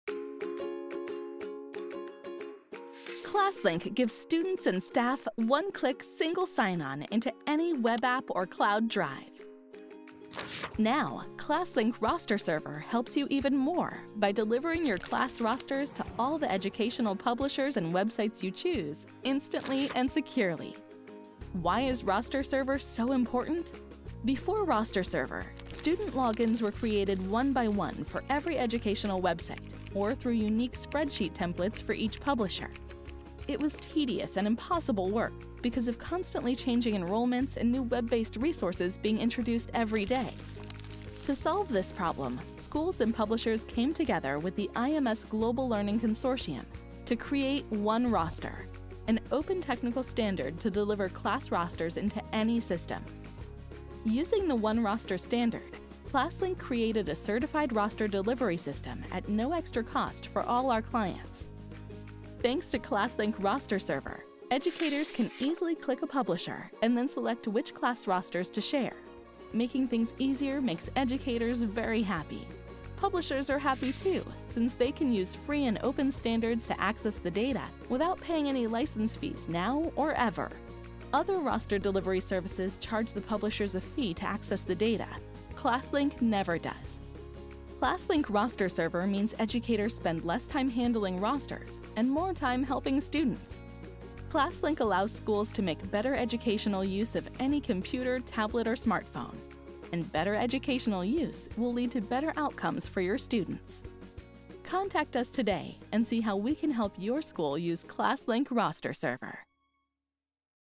Why Is Roster Server Important?
ClassLink delivers class rosters to applications using the OneRoster standard from IMS Global. ClassLink Roster Server is the secure and timely delivery of online education resources.